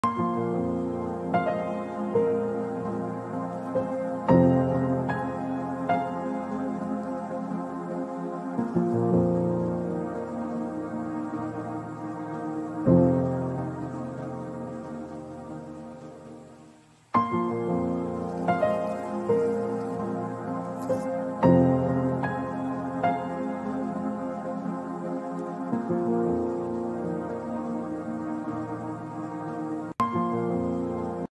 Collection Gustav Klimt de Robert Kaufman
Les fans de Gustav Klimt seront en joie devant cette toute nouvelle collection 2022 qui n'a rien a envier à la précédente , elle est absolument sublime !